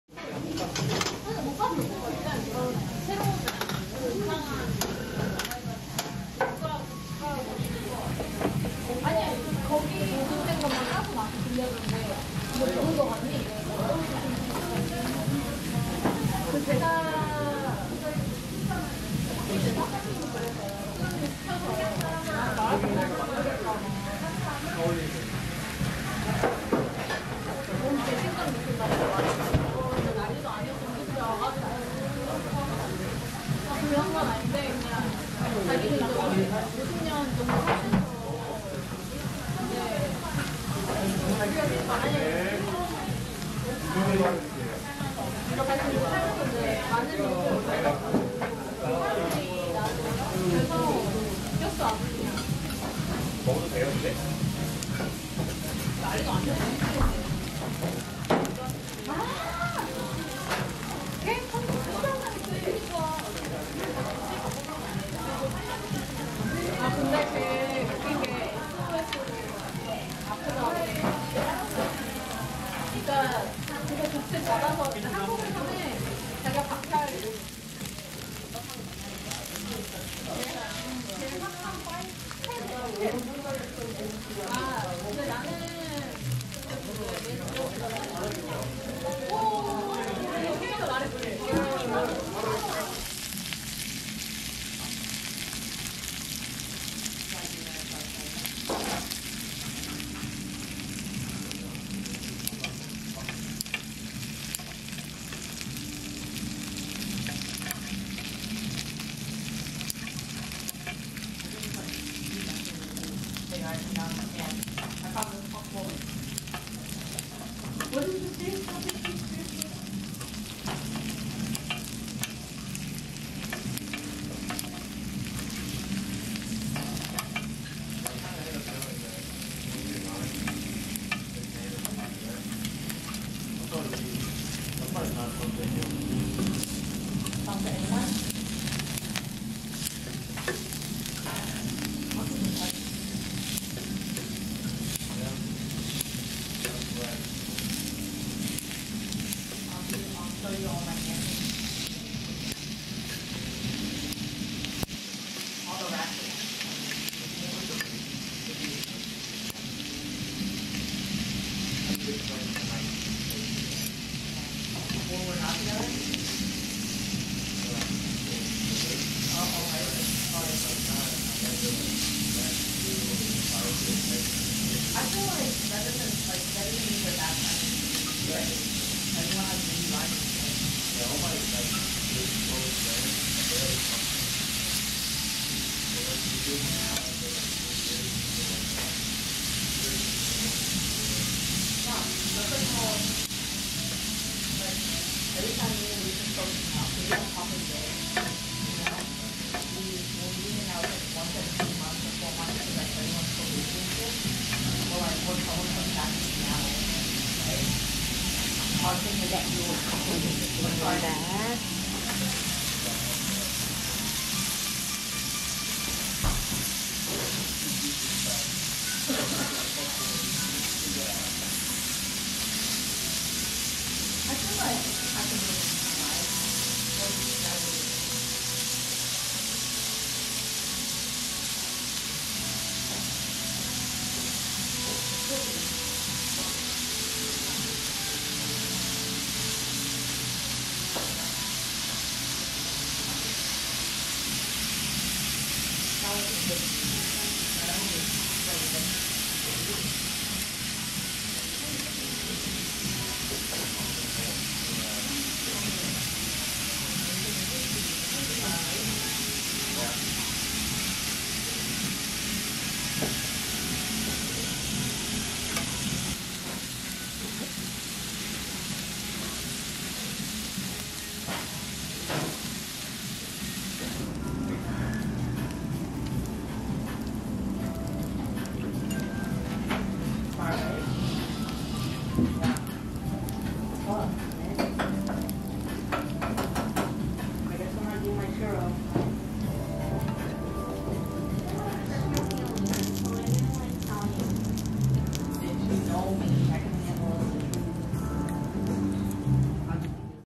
[JMT] 치즈폭탄 화산 계란 볶음밥 / Volcano Cheese Egg Fried Rice
볶음밥 1인 4000원 (For 1 Person USD 3.5)
삼겹살 1인 14000원 (For 1 Person USD 12.4)

서울 마포구 잔다리로 70 한림돈가
70, Jandari-ro, Mapo-gu, Seoul, Republic of Korea